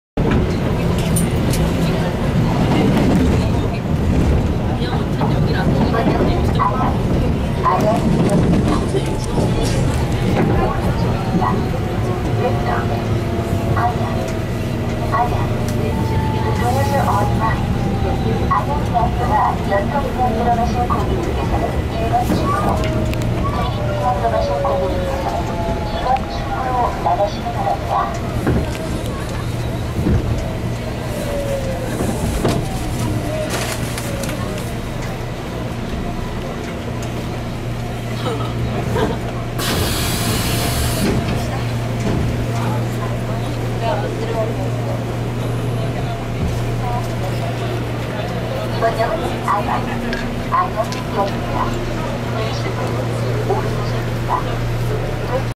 Riding a Seoul Metropolitan Subway System train - South Korea 2012.
Riding a Seoul Metropolitan Subway System train in South Korea 2012.